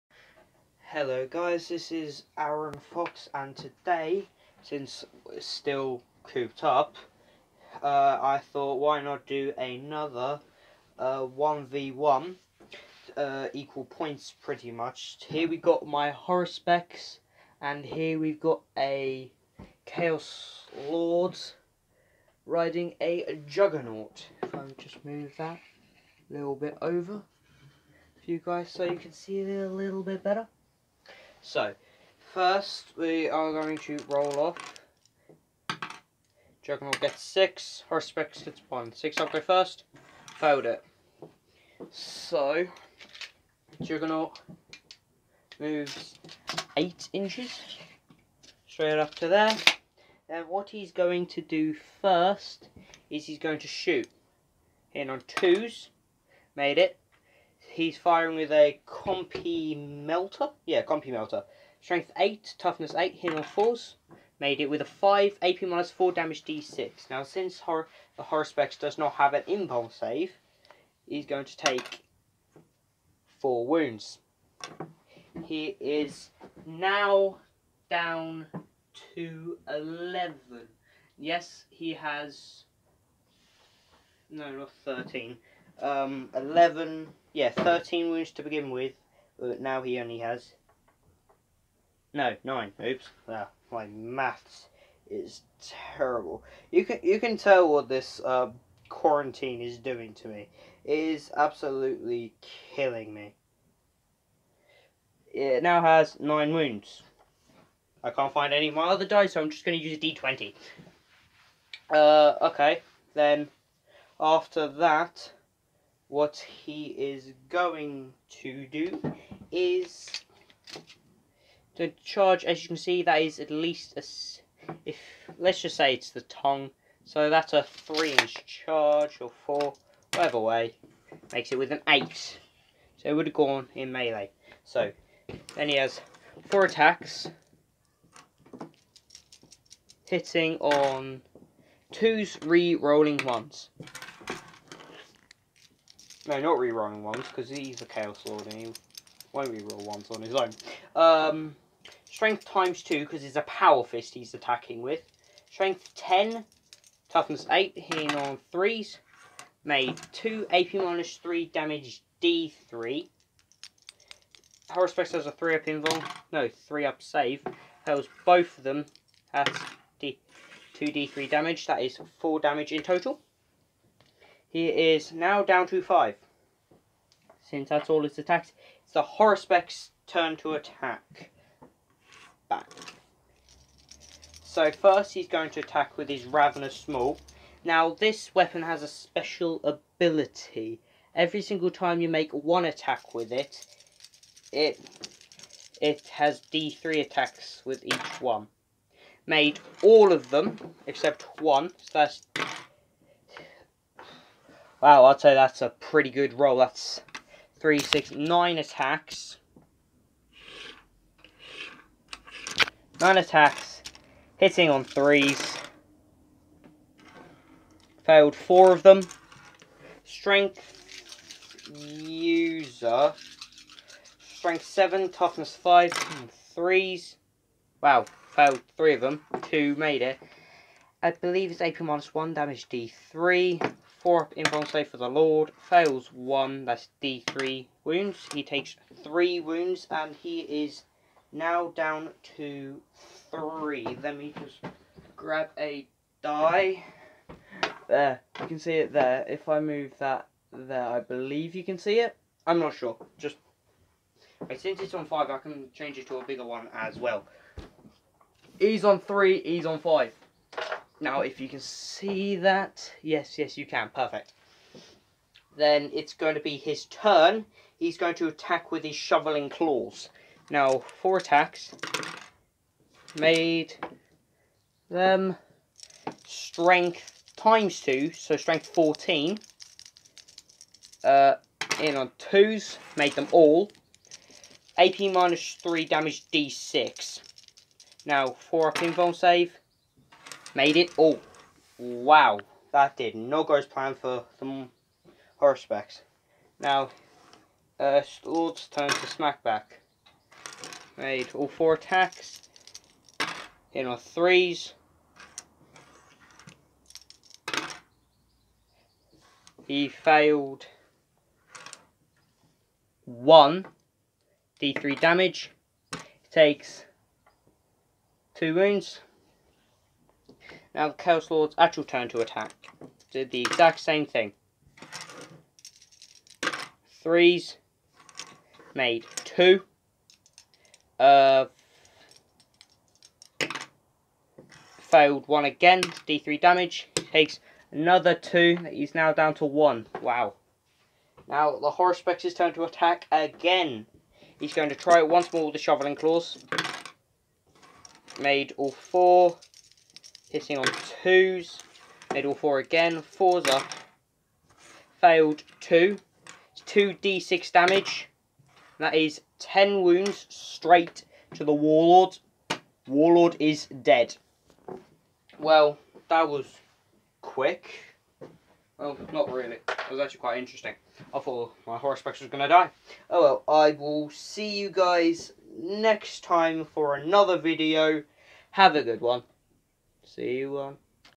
Who would win? Khorne chaos lord vs hauruspex
in this video Warmaster Aaron's chaos lord on a juggernaut of Khorne faces off against his Tyranid haruspex, both giant beasts in combat and both a force to be reckoned with, it's unstoppable force versus immovable object, who will win? watch to the end to find out!